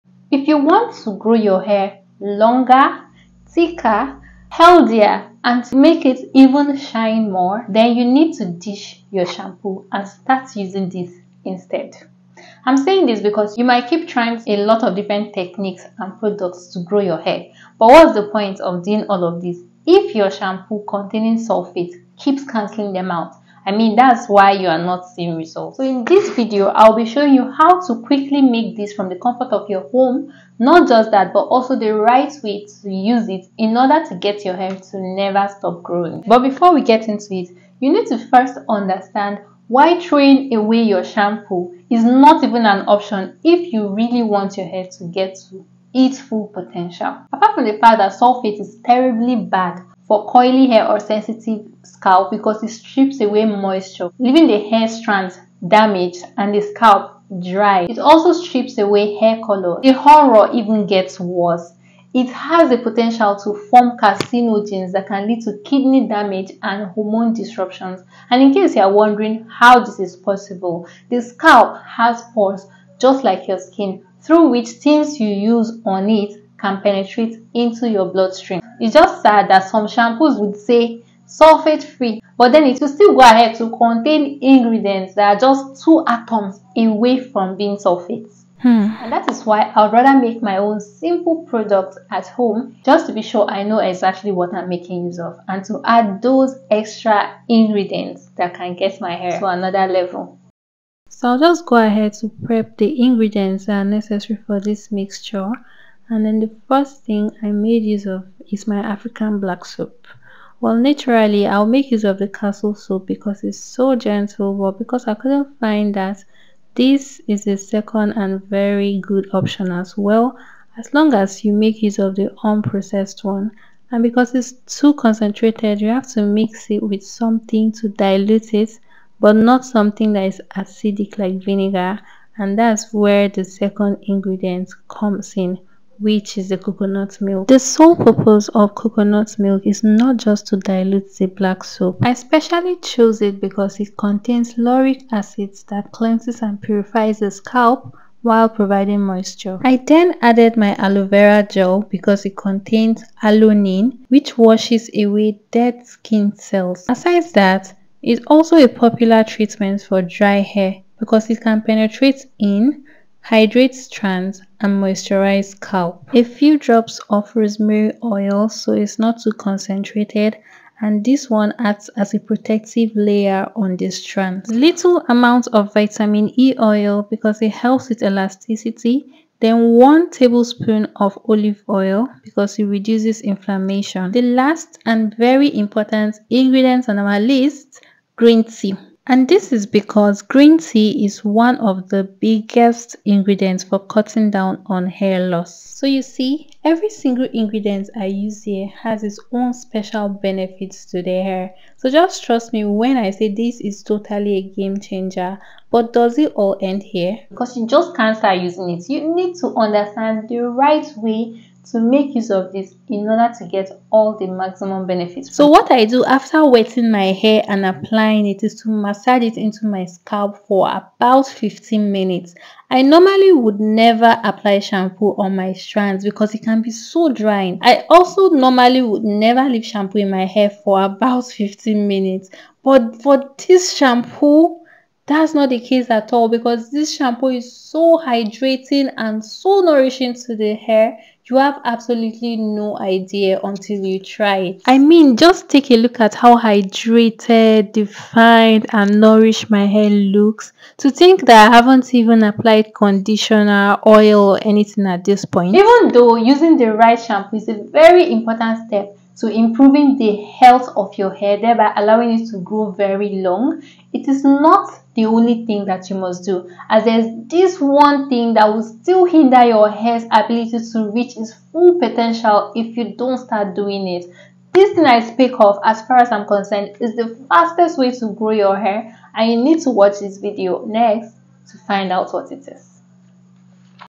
Use this instead and your hair will never stop growing
In this video i revealed and explained how badly the shampoo we use keeps hindering our hair from growing fast. This shampoo ofcourse is shampoo containing sulphate, so i offered a perfect alternative that will not only prevent this effect but will still go ahead to grow your hair even faster. i showed a simple way to make this from the comfort of your home, not just that but also the right way to use it in order to get your hair to never stop growing.